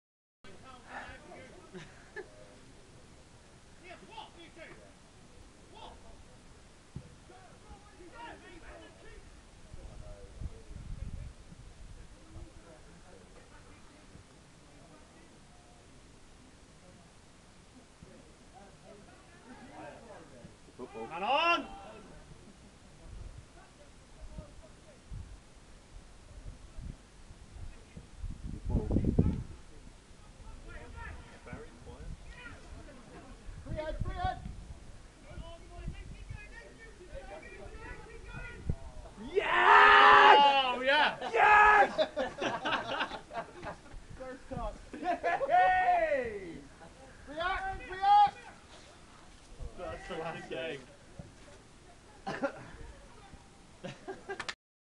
The Duchy League Div3 Game, St Stephen Res 4 - 0 St Dennis 3rds
match highlights  St Stephen keeper Steve Raven showboating lol in the 1st minute.